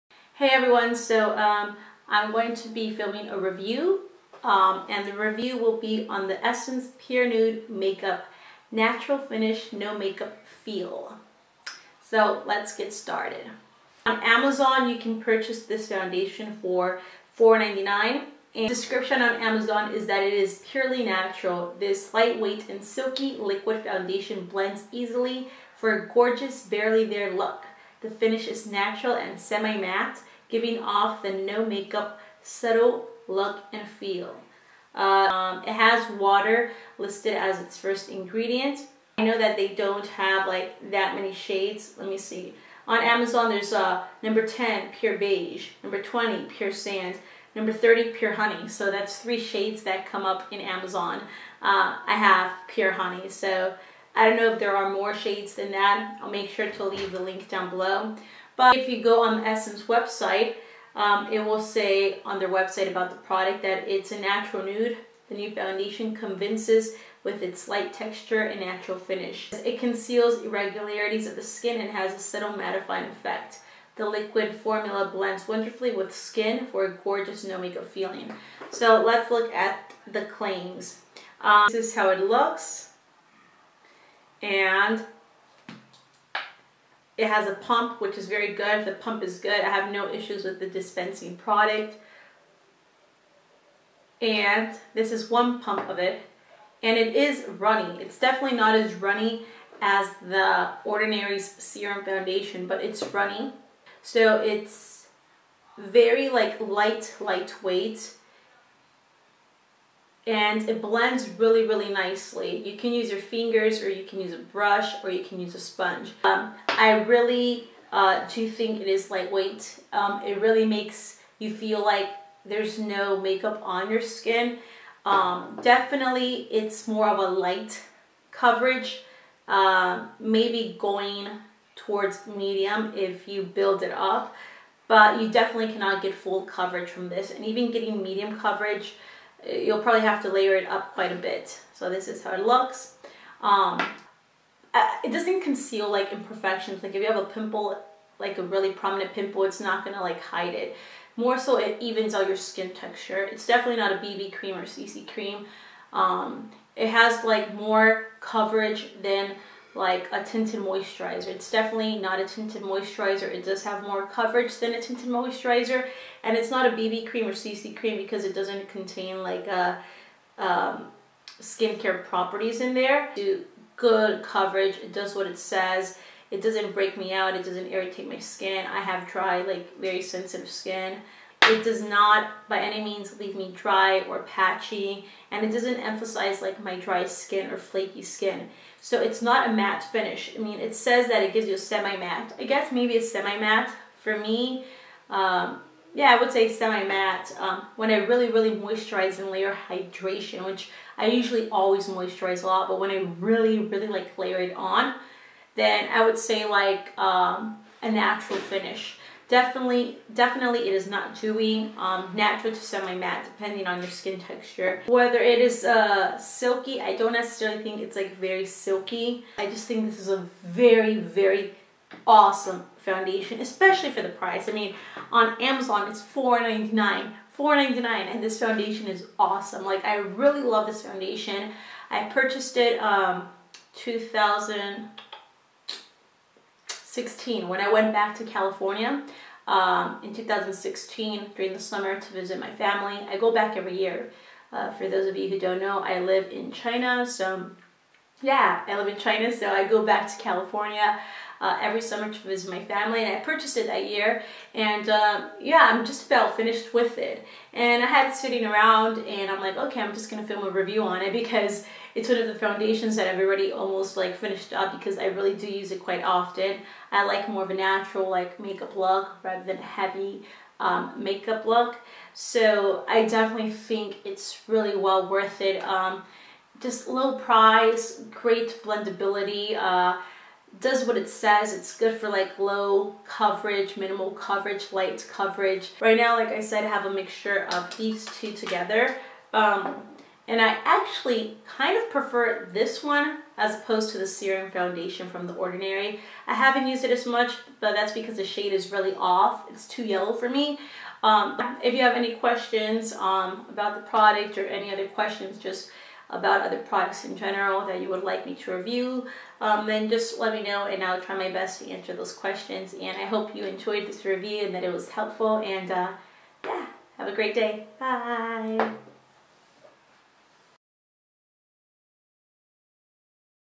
Awesome Lightweight Foundation: Essence Pure Nude Review
Hey everyone. I have used the Essence foundation for a while now, and i have been loving it. Here is my review on it.

Product Mentioned :